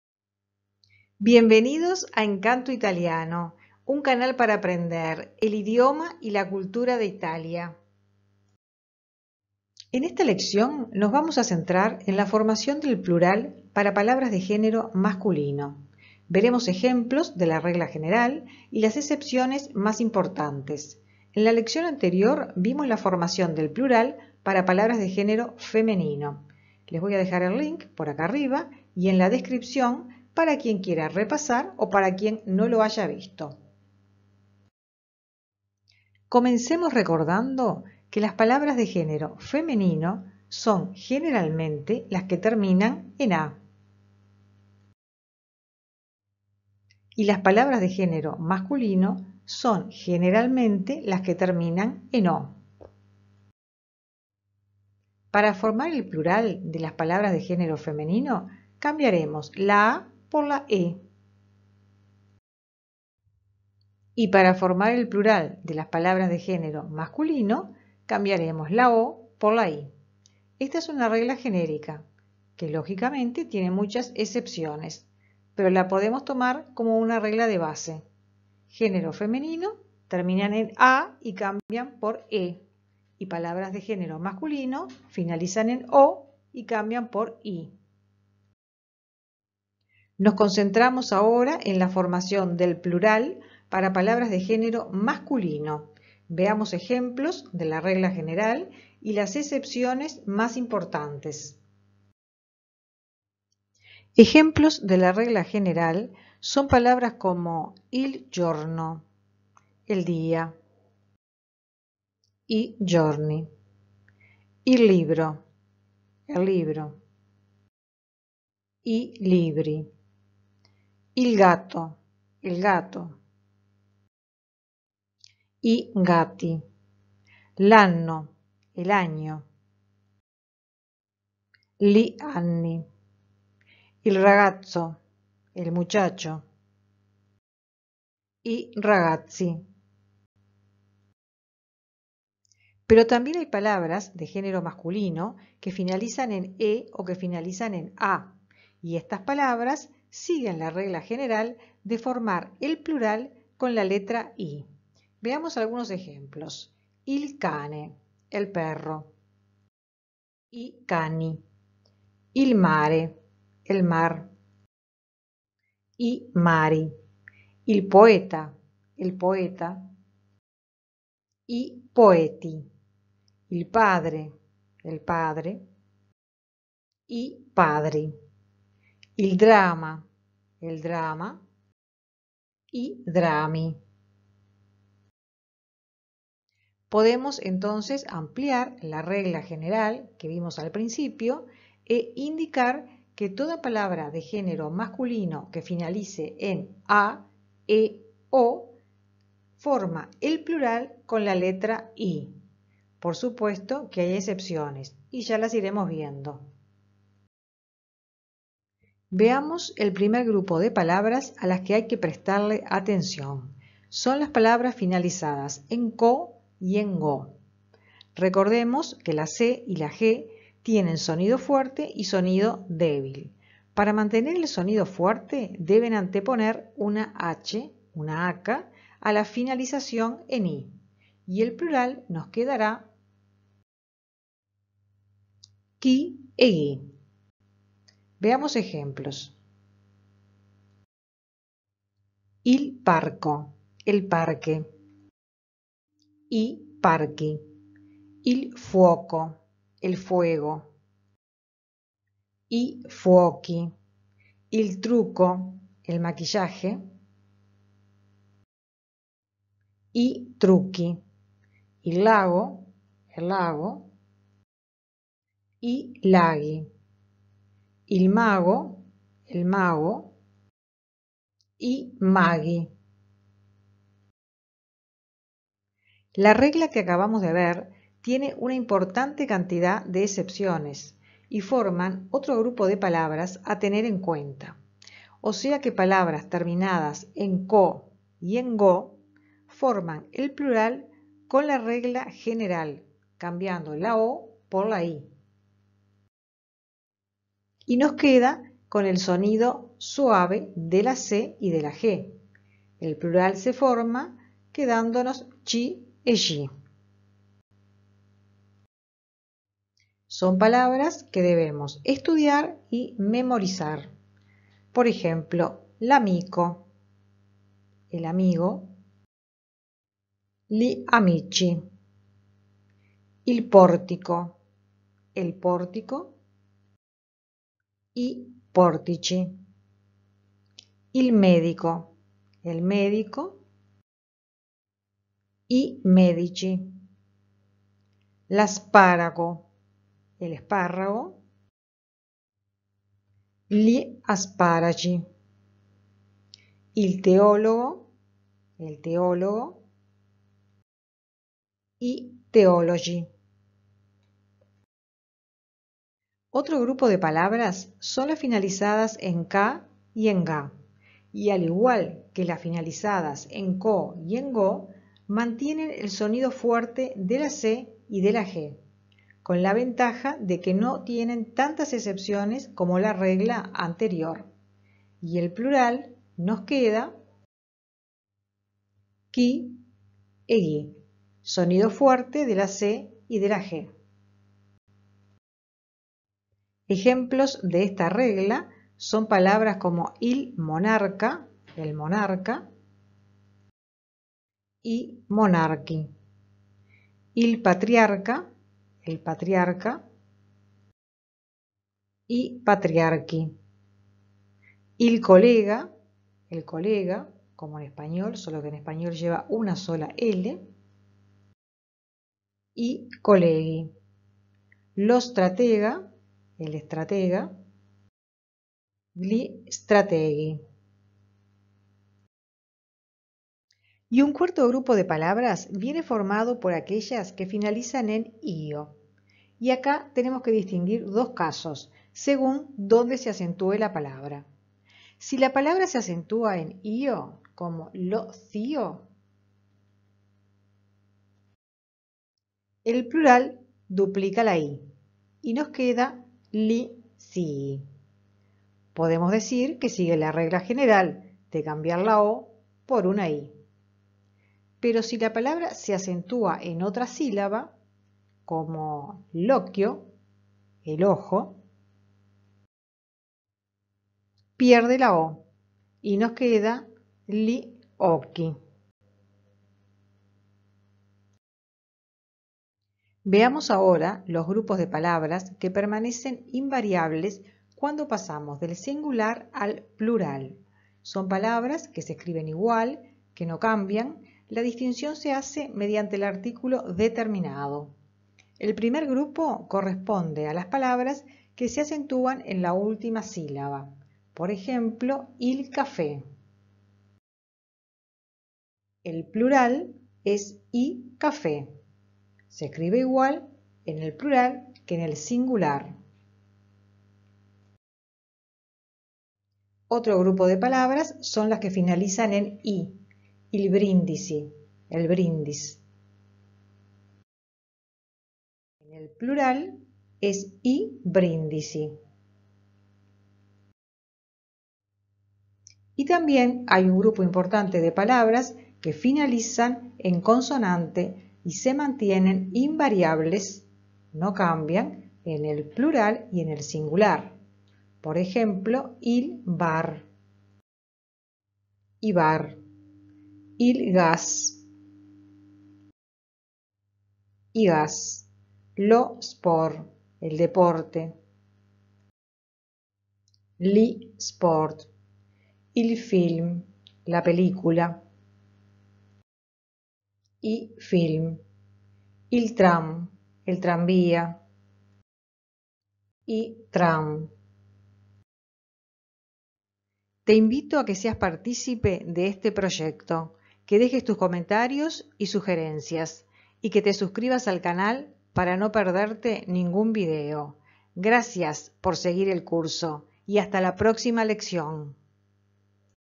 52-Formación del plural en palabras de género masculino. Formazione del plurale dei nomi maschili.
¿Sabes como se forma el plural en las palabras de género masculino?
Conoces las excepciones más importantes?
En este video te lo explicamos de manera fácil y amena.